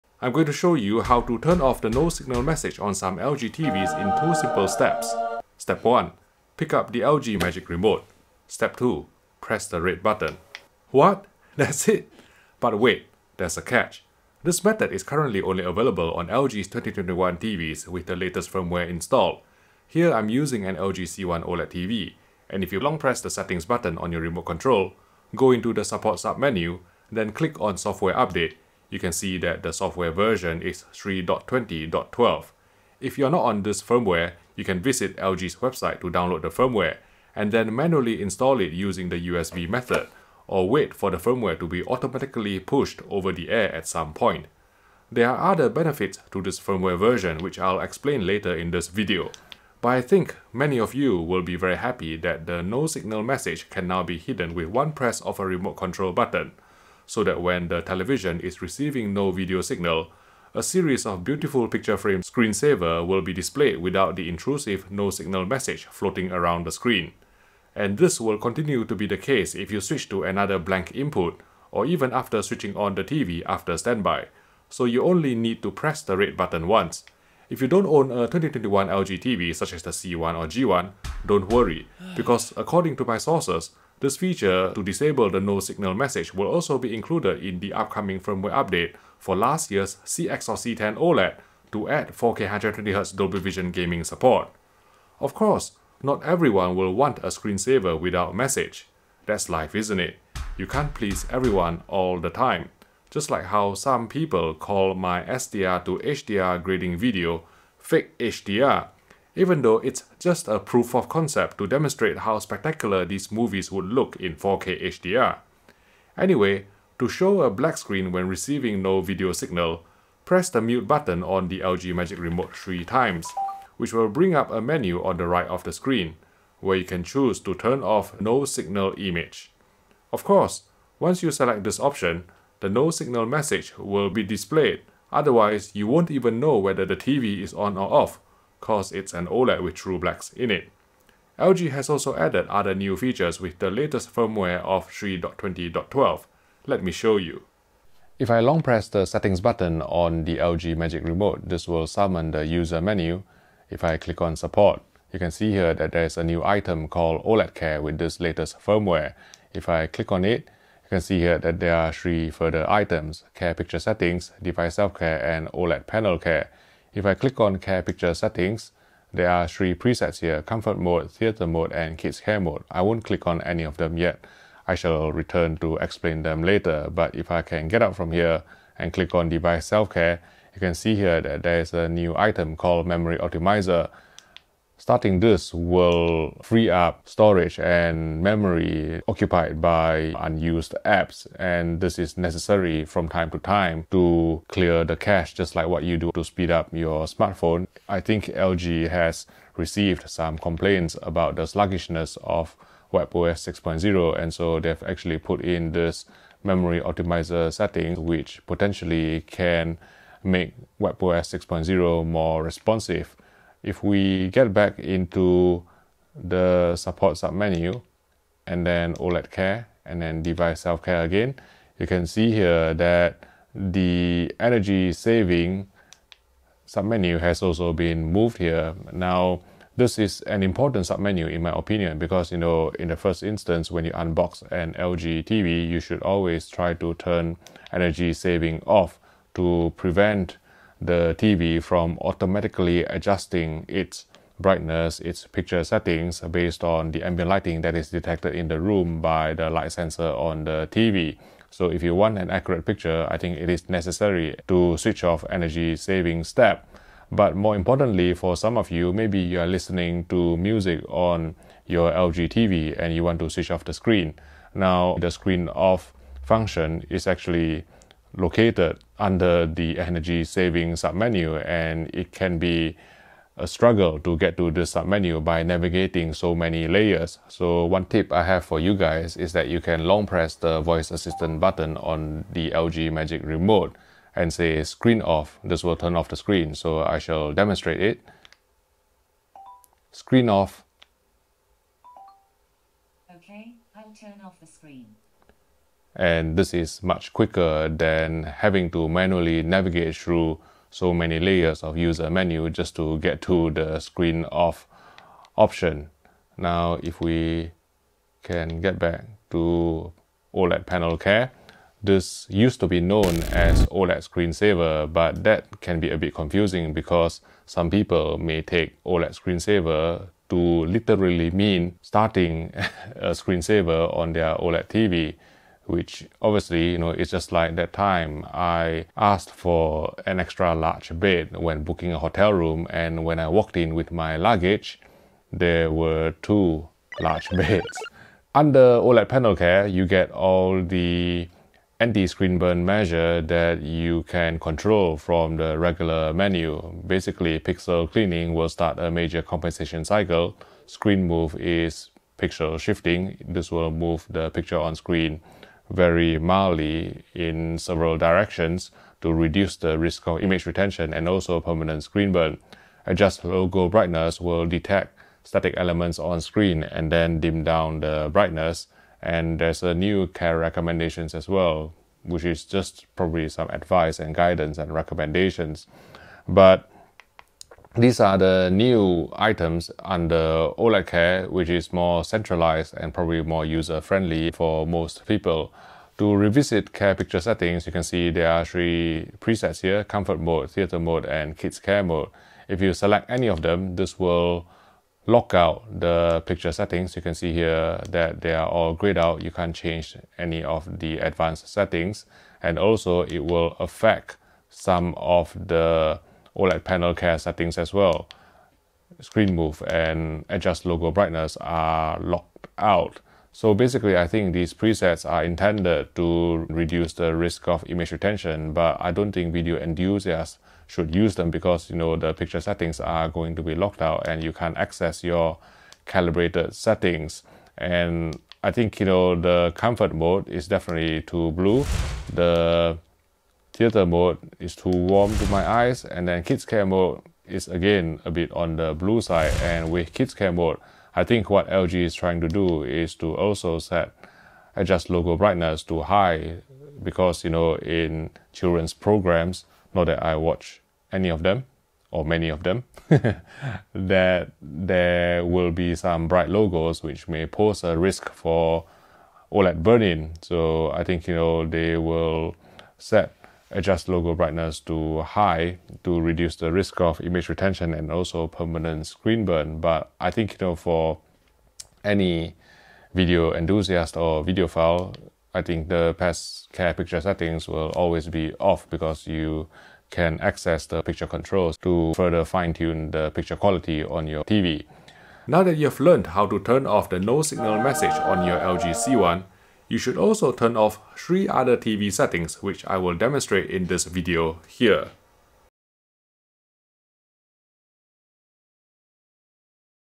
FINALLY! LG's No Signal Message Can Now Be Turned Off on C1 (& CX Soon)
You can now turn off the "No Signal" message on the LG C1 OLED, which is a feature added by the latest firmware update of 03.20.12 (the last two digits may vary by region). According to our sources, this function will also be included in a forthcoming software update for last year's LG CX OLED to add 4K 120Hz Dolby Vision gaming support.

Other features added by this firmware include a new "OLED Care" submenu, as well as a "Memory Optimiser" feature to improve the responsiveness of WebOS 6.0 by clearing unused apps, thus freeing up memory & storage.